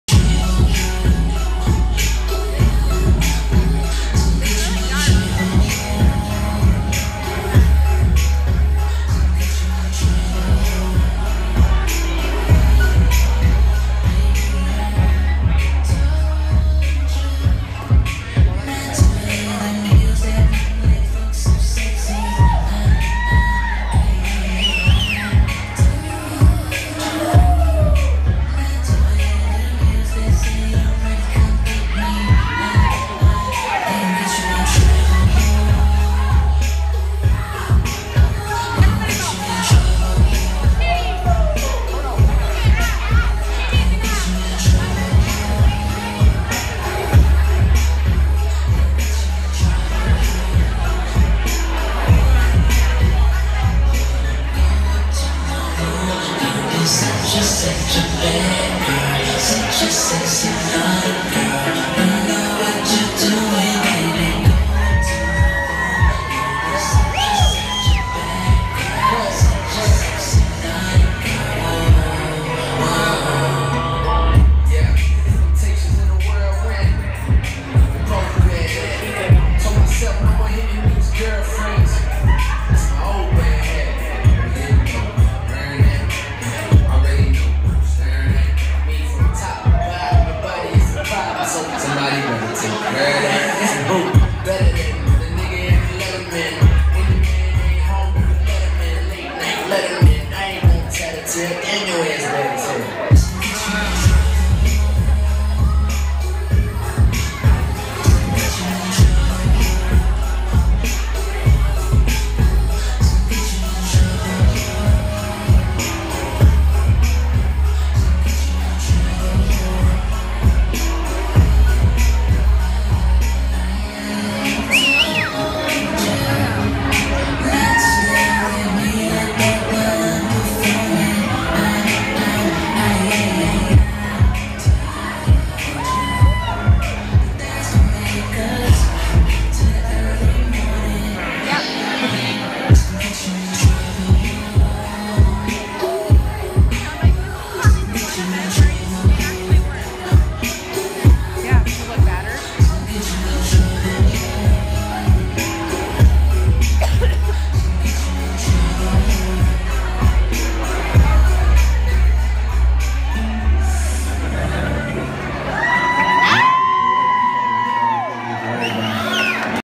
Pole ambiance at Salon 7, Reno, Oct 2015
This Halloween party was in the salon; there was no professional lighting; video is dark